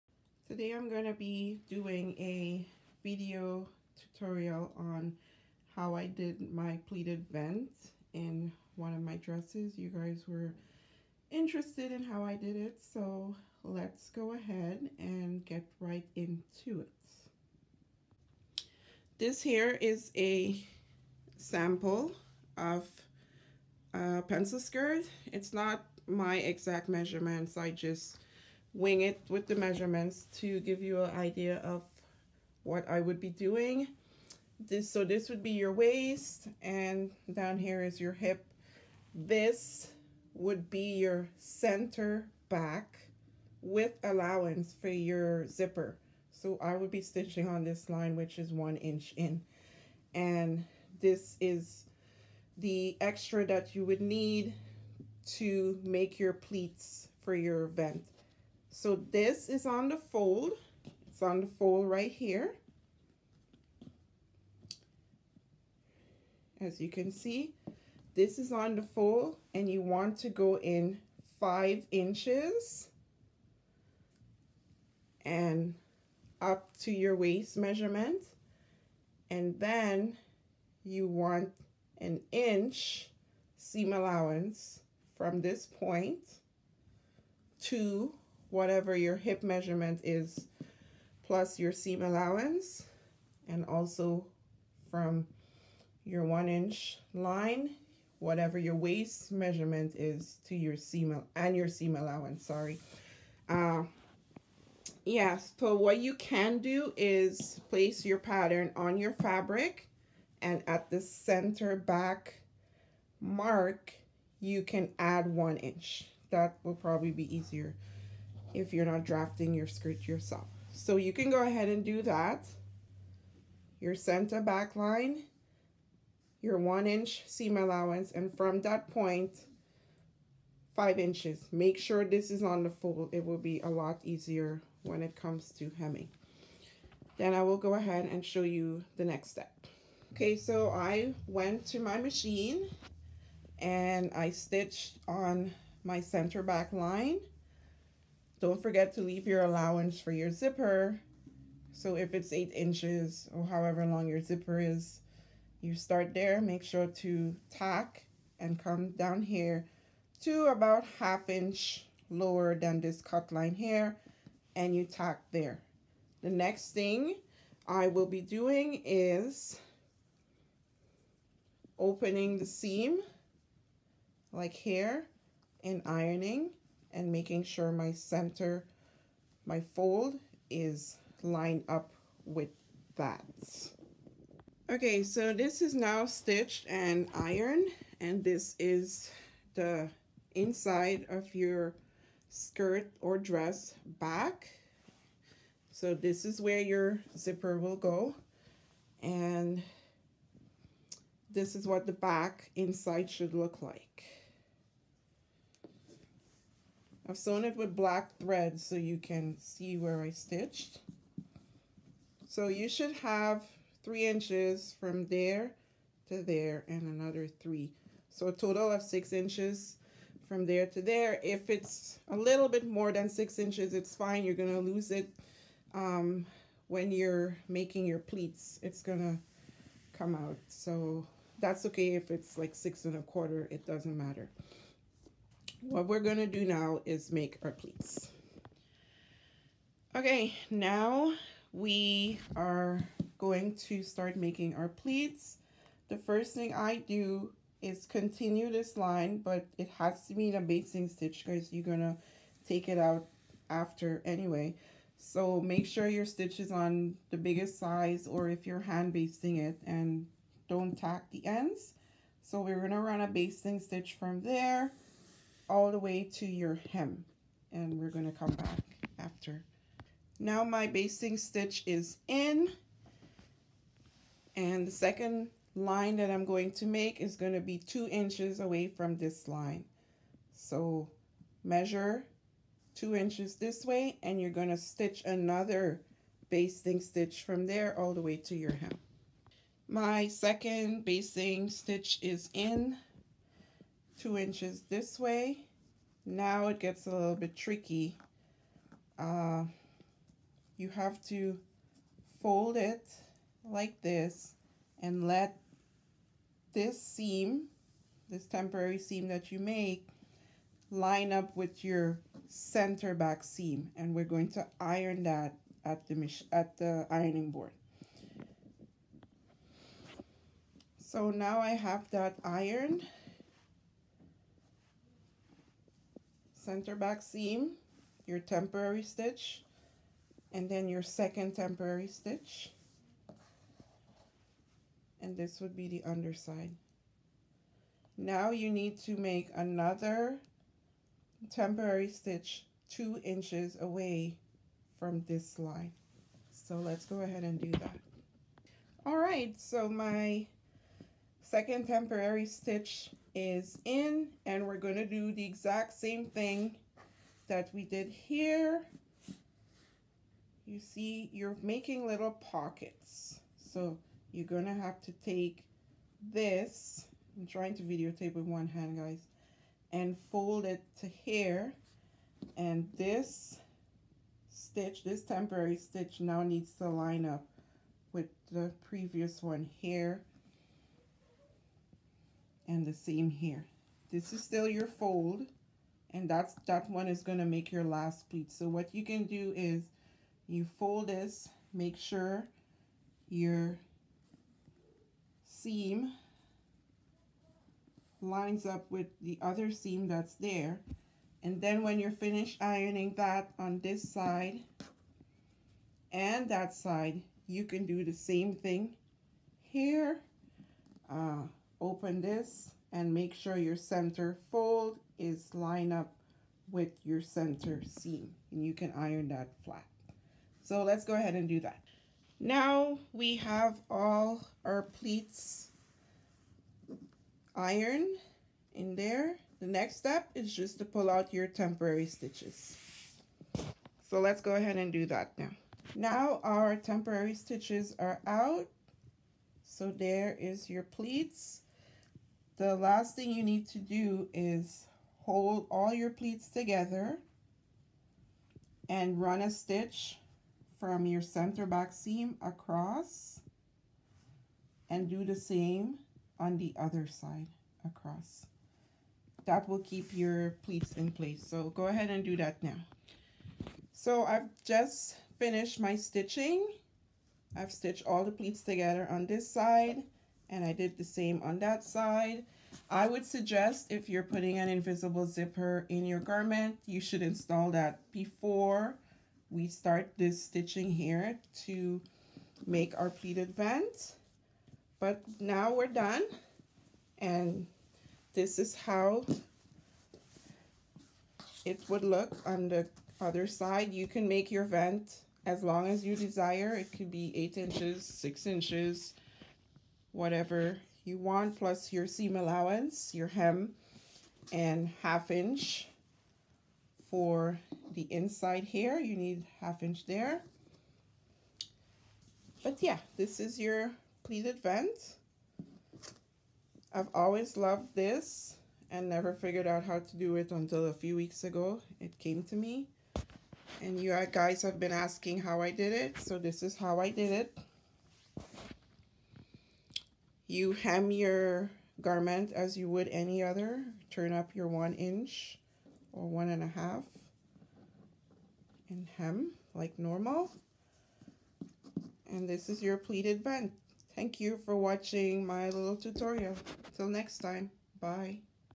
Pleated Vent Tutorial
Want to add some sass to the back of your pencil skirt or dress? Follow along on my tutorial on how to make a Pleated Vent.